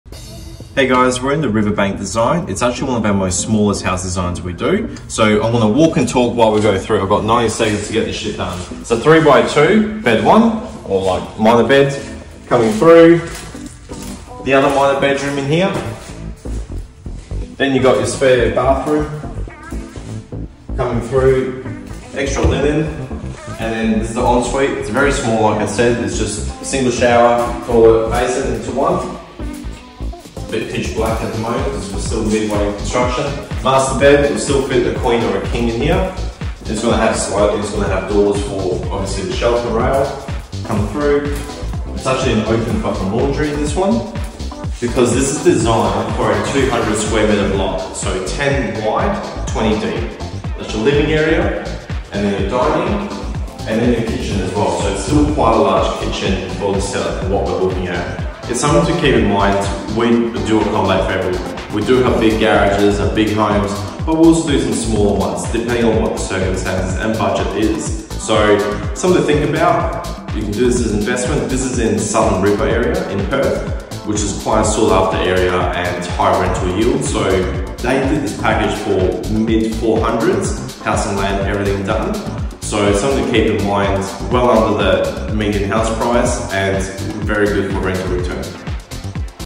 The Riverbank design
We’re back with another client home walkthrough.

This walkthrough is of The Riverbank design built in Southern River, Western Australia.

This design is suited to fit on a 10m wide block or larger and 20m deep (200m2 block).

This design features: 3 bedrooms, 2 bathroom and a double car garage with a store section. This is an open plan living design and the master has a single shower, single basin and sliding robes.

We hope you enjoy this walkthrough and gained some inspiration for your design. If you would like to see the full video, head over to our YouTube page!